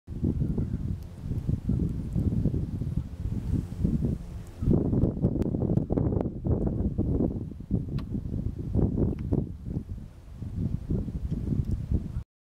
Wilson's Phalarope's acting more strangely than usual Sept.27/13, Morse, SK
Phalaropes are famous for spinning in the water, but here's a behavior I haven't seen before - they're in shallow water catching tiny bugs that were swarming in the air, near Morse, Saskatchewan